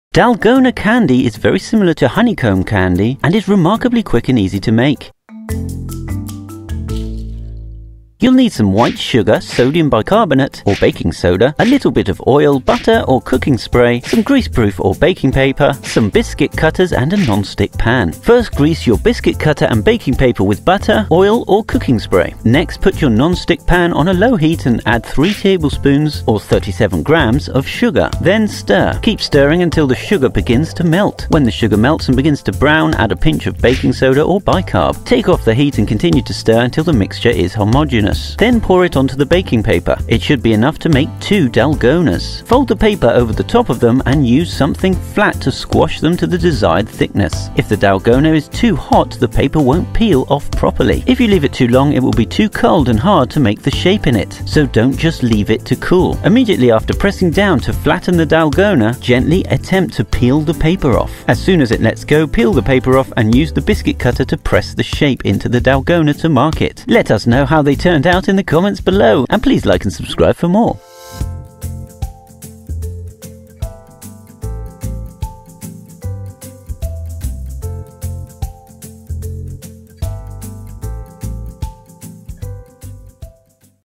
How To Make Dalgona Candy!
Dalgona Candy is very similar to honeycomb candy and is remarkably quick and easy to make. You'll need some white sugar, sodium bicarbonate or baking soda, a little bit of oil, butter or cooking spray, some greaseproof or baking paper, some biscuit cutters, and a non-stick pan.

First grease your biscuit cutter and baking paper with butter, oil, or cooking spray.
Next, put your non-stick pan on a low heat and add 3 tablespoons or 37grams of sugar, and stir.
Keep stirring until the sugar begins to melt.
When the sugar melts and begins to brown add a pinch of baking soda or bi-carb.
Take off the heat and continue to stir until the mixture is homogenous.
Then pour it onto the baking paper. It should be enough to make 2 Dalgona.

Fold the paper over the top of them and use something flat to squash them to the desired thickness.

If the Dalgona is too hot the paper won't peel off properly but, if you leave it too long it will be too cold and hard to mark the shape in it, so don't just leave it to cool.
Immediately after pressing down t flatten the Dalgona, gently attempt to peel the paper off, as soon as it lets go, peel the paper off and use the biscuit cutter to press the shape into the Dalgona to mark it.

let us know how they turned out in the comments below.